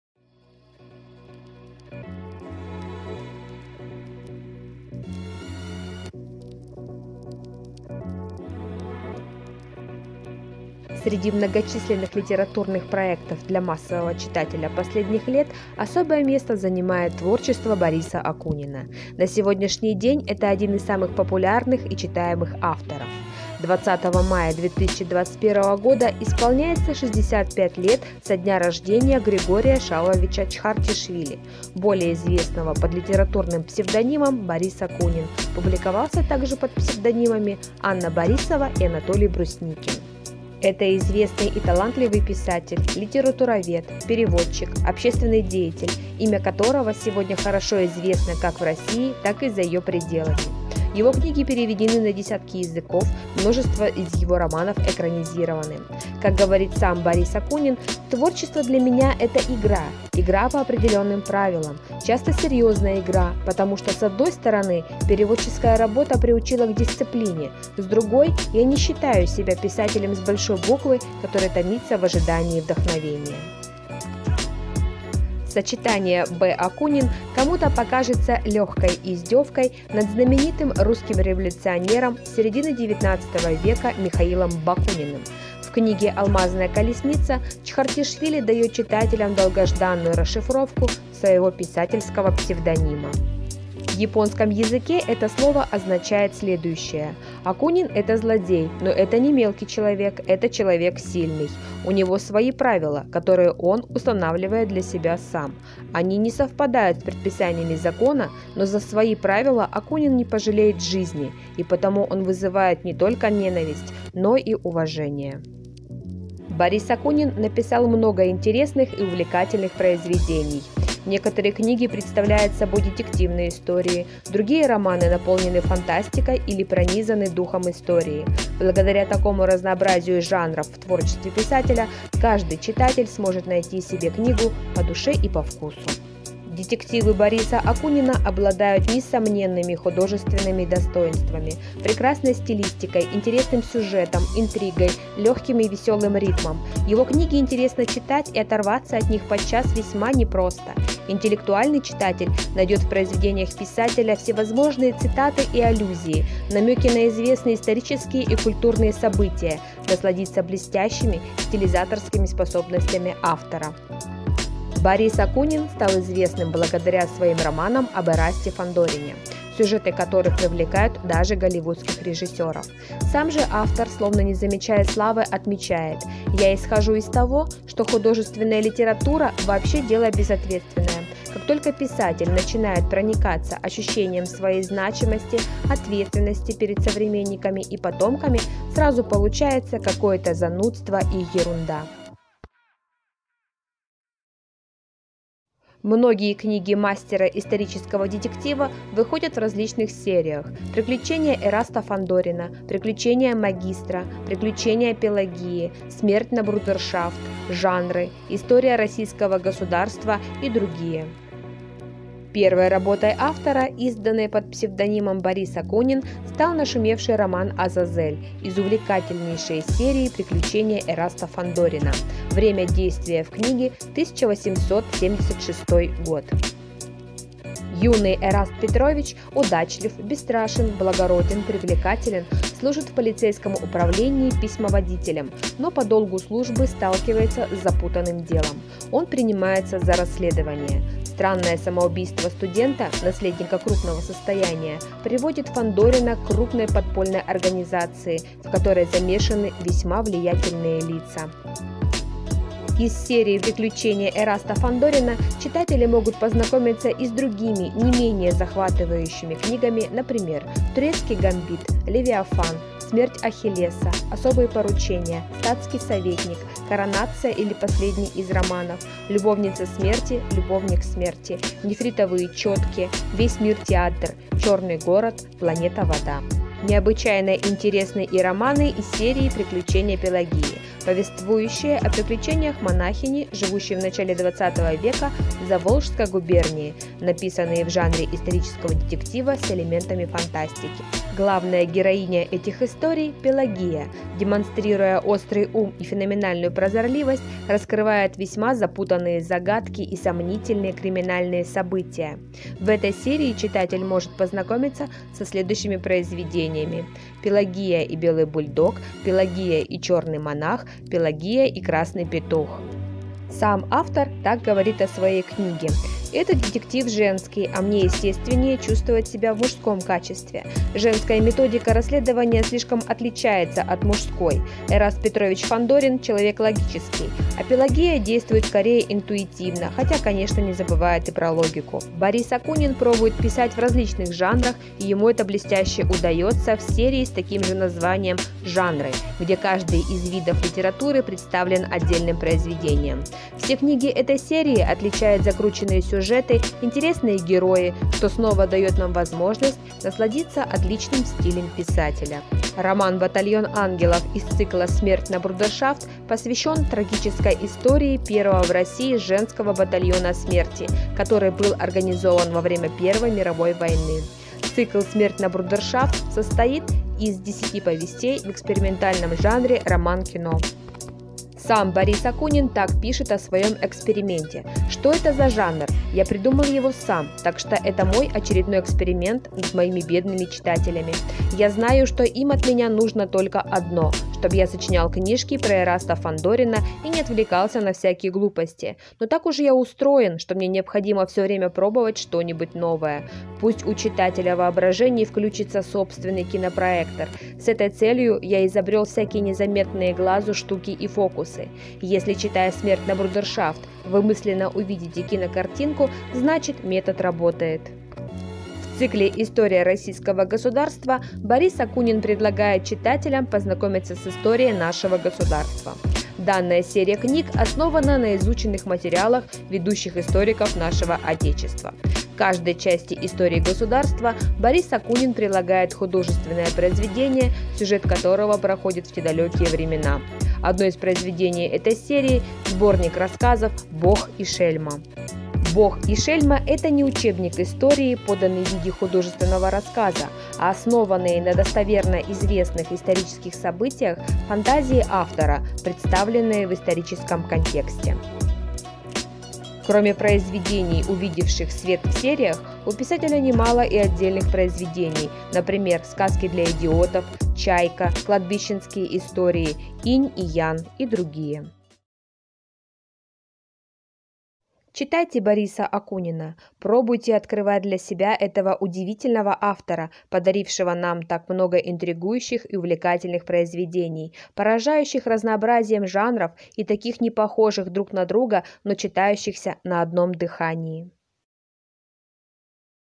Литературные маски Бориса Акунина
Борис Акунин – российский писатель грузинского происхождения, настоящее имя которого Чхартишвили Григорий Шалвович. Самые известные его произведения – цикл детективов о талантливом дворянине Эрасте Фандорине. Предлагаем вашему вниманию информ-досье по творчеству писателя.